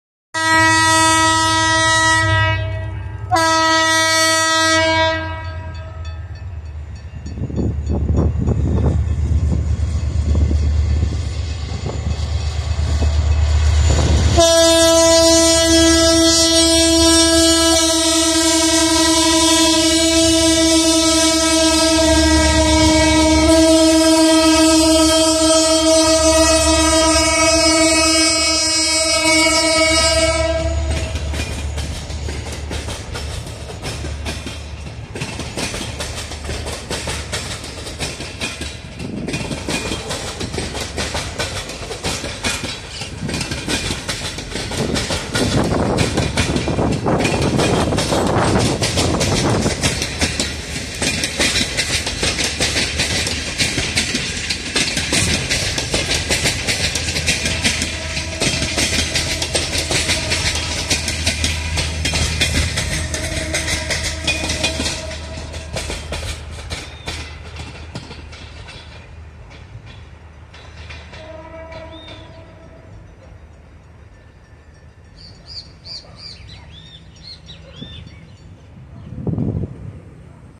TA- Tren N°267 con GT22 y CKD saliendo de Villa María rumbo a Córdoba
Tren N°267 de Trenes Argentinos a cargo de las locomotoras GT22CW A923 y CKD 8H 0007 saliendo de Villa María rumbo a Córdoba. 04/10 07:51 AM.